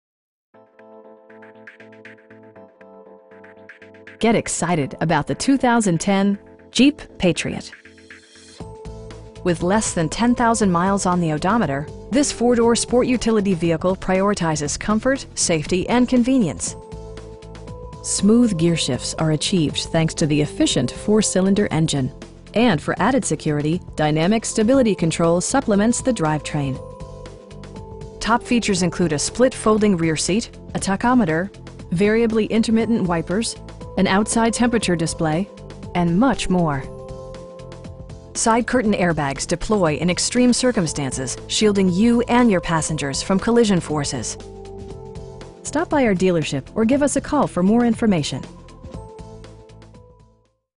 2010 Jeep Patriot Sport in Lake Wales, FL 33859
Sorensen & Schade Chrysler Dodge Jeep Ram

Get excited about the 2010 Jeep Patriot. With less than 10,000 miles on the odometer, this 4 door sport utility vehicle prioritizes comfort, safety and convenience. Smooth gearshifts are achieved thanks to the efficient 4 cylinder engine, and for added security, dynamic Stability Control supplements the drivetrain. Top features include a split folding rear seat, a tachometer, variably intermittent wipers, an outside temperature display, front bucket seats, tilt steering wheel, and much more. Side curtain airbags deploy in extreme circumstances, shielding you and your passengers from collision forces. We pride ourselves in consistently exceeding our customer's expectations. Stop by our dealership or give us a call for more information.